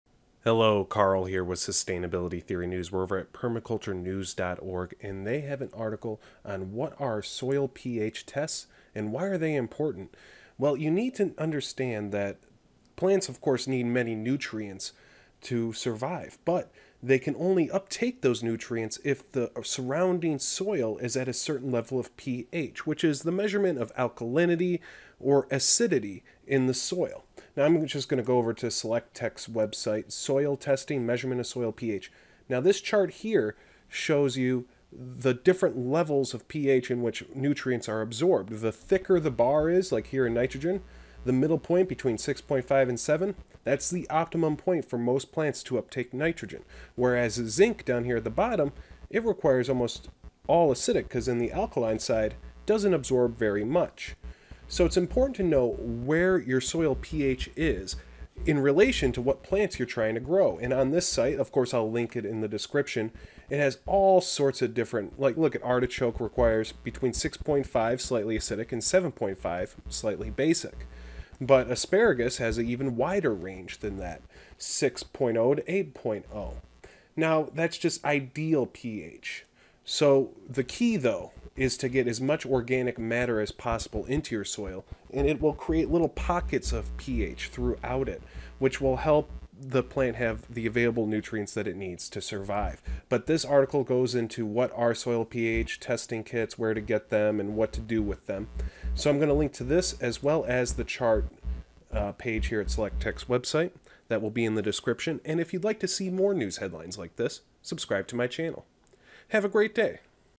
Why Soil PH Is Important For Plant Growth
Understanding why plants need proper PH balance in order to grow is vital is you want the most productive farm or garden possible. These articles include how to use soil PH testing kits, a chart of plant nutrients and what PH they are most absorbed, and a list of plants with their ideal PH range.